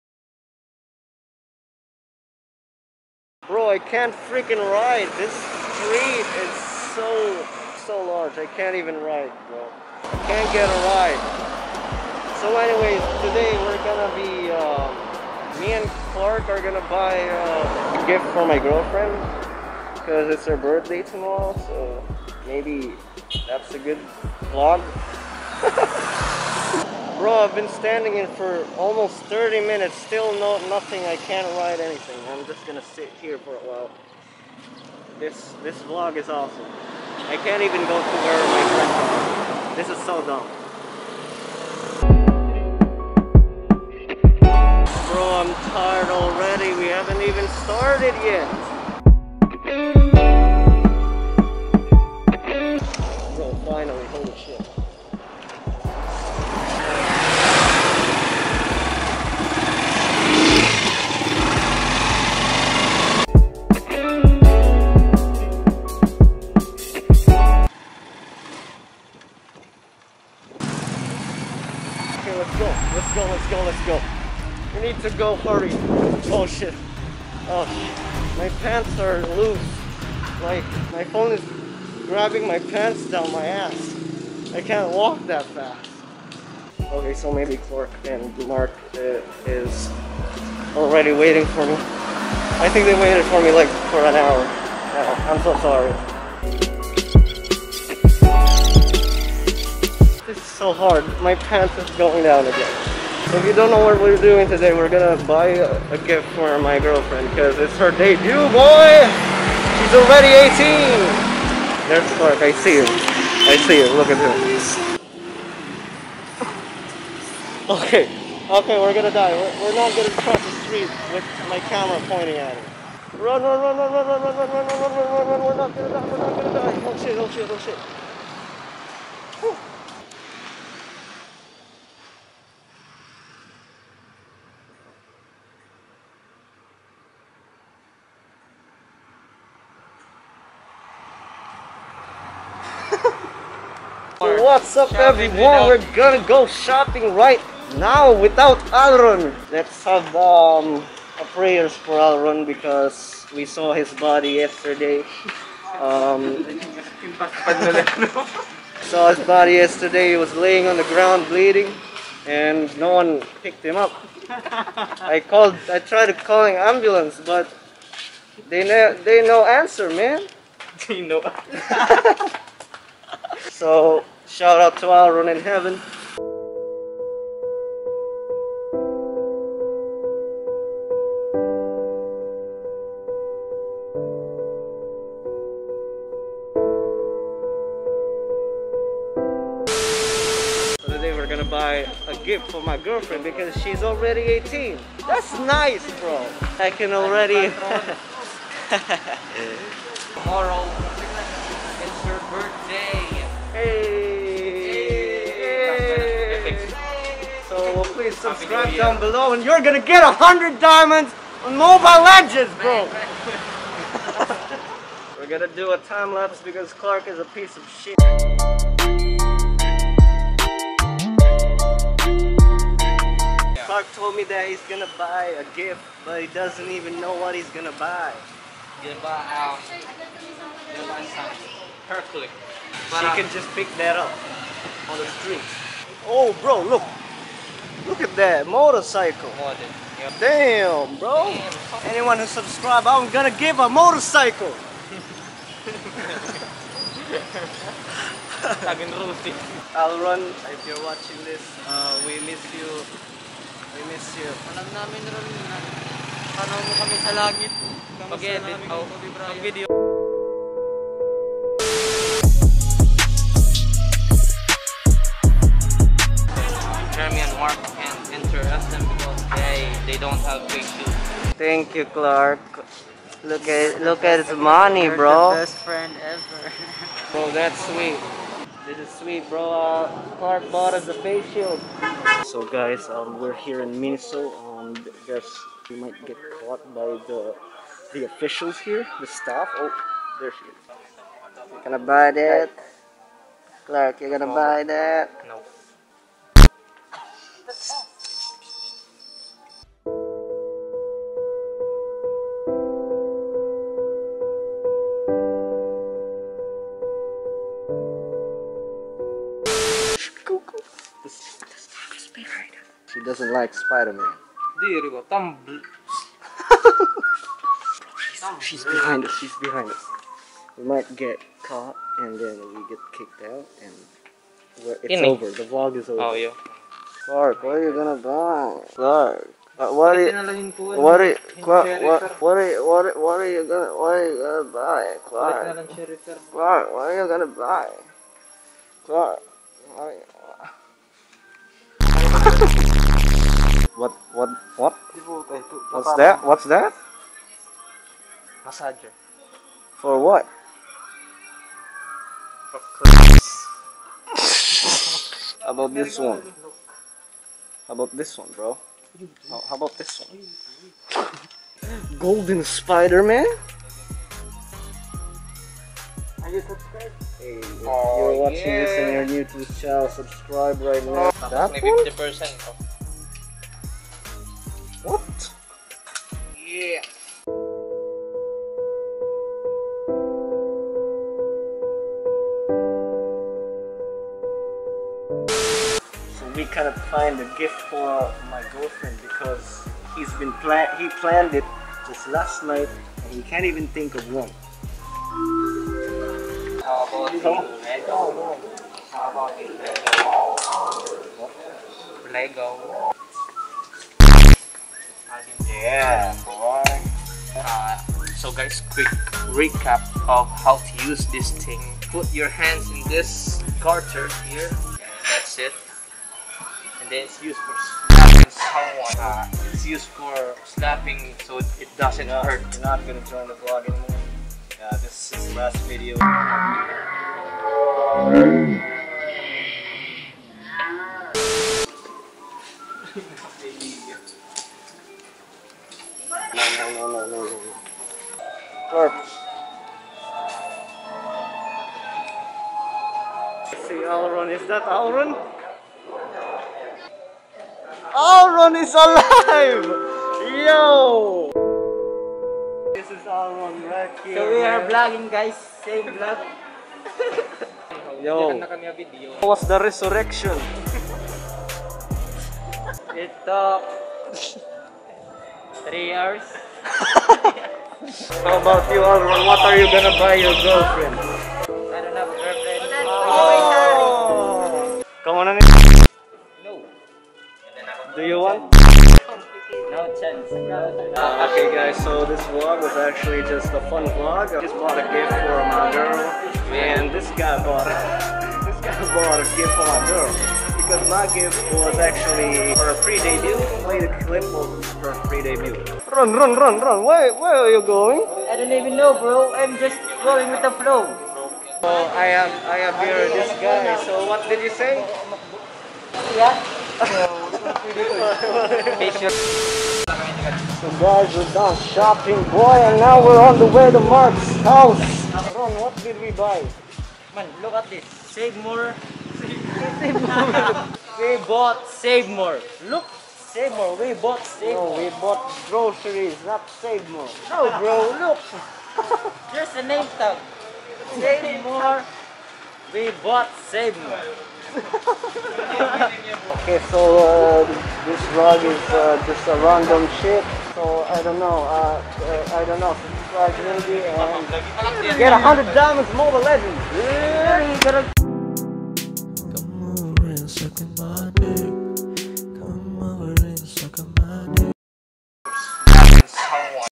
A Shopping Vlog...
We went shopping to buy a gift for My Girlfriend for her Debut!!
It was fun lol